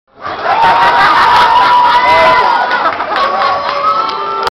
Bisounours magicarpe - Paris manga février 2010
Benkei faisant le Magicarpe après que Shade l'ai fait aussi